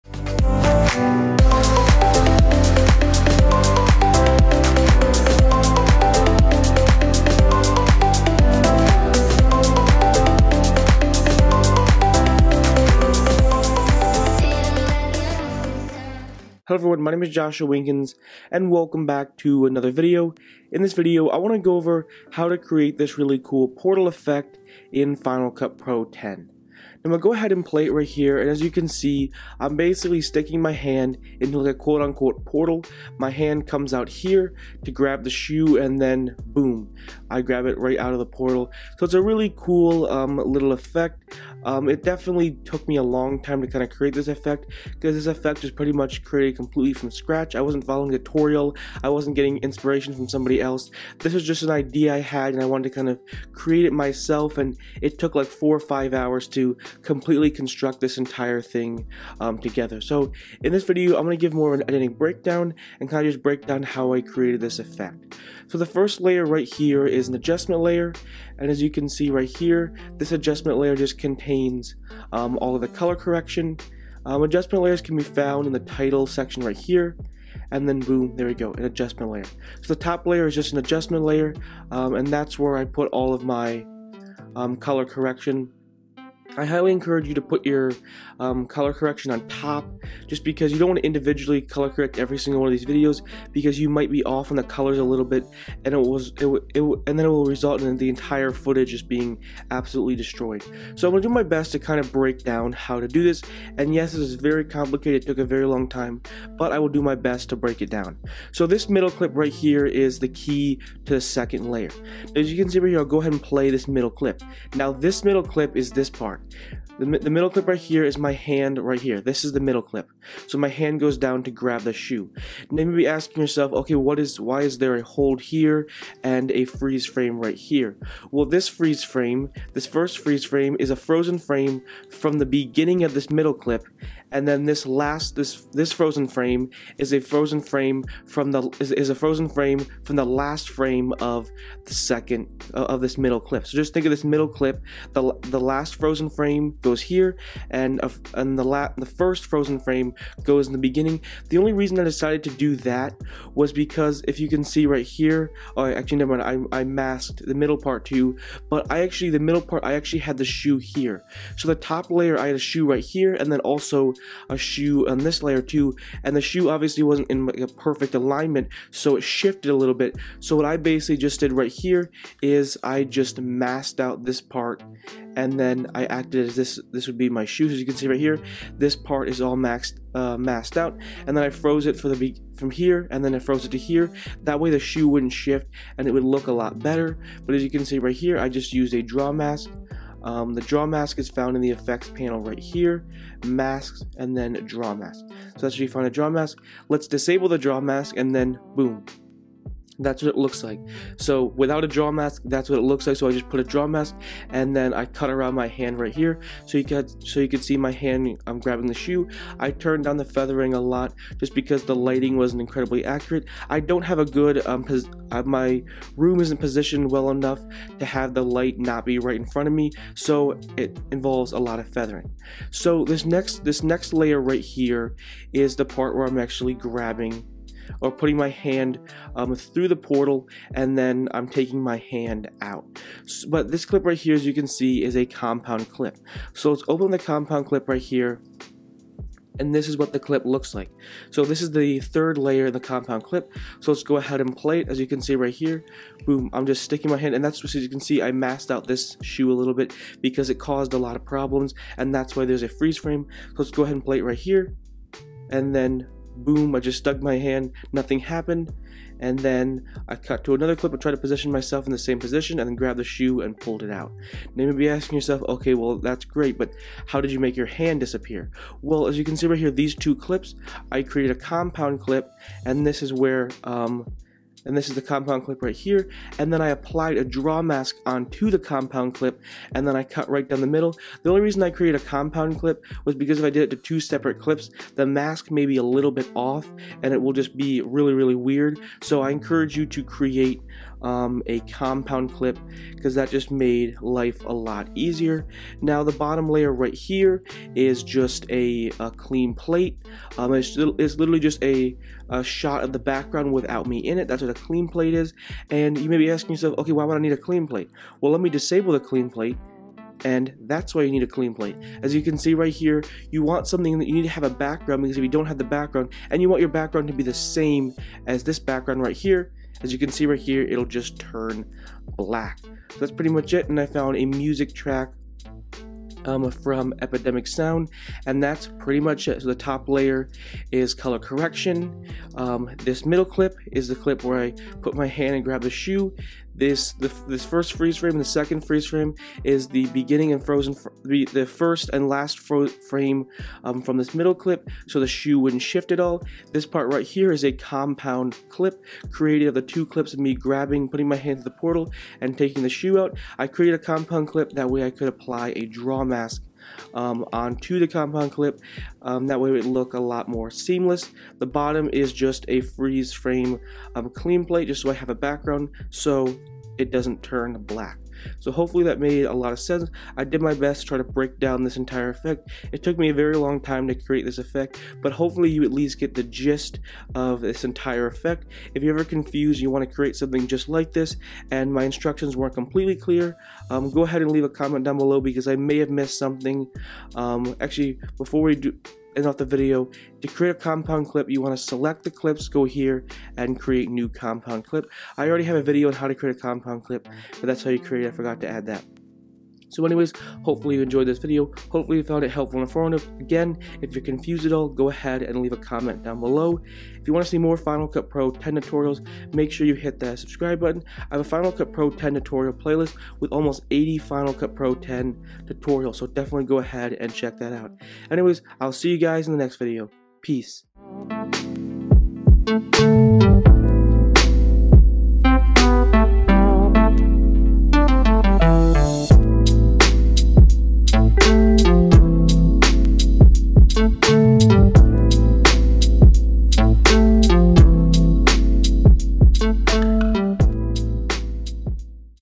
Final Cut Pro X Portal Effect Tutorial
In this tutorial, I will break down how to create a Portal Effect in Final Cut Pro X.